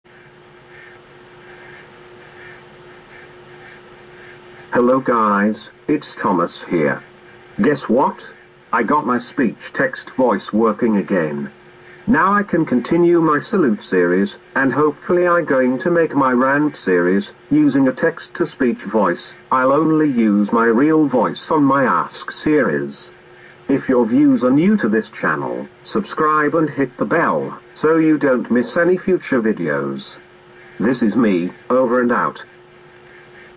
I Got My Speech Text Voice Working Again
Now I can continue my salute series, and hopefully I going to make my rant series, using a text to speech voice, I'll only use my real voice on my ask series.